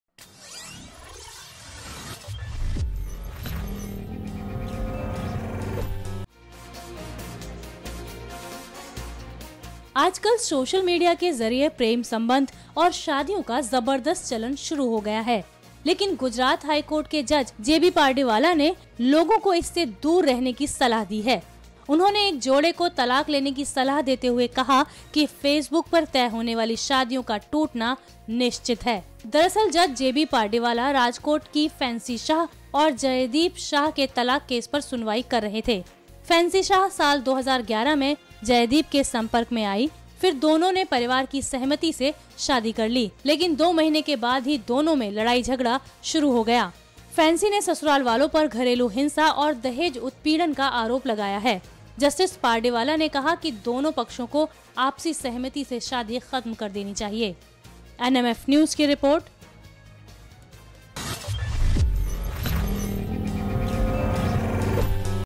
Marriages पर Gujrat High Court की टिप्पणी, नहीं चलती Facebook वाली शादी
Gujrat High Court said that marriages through facebook must be failed.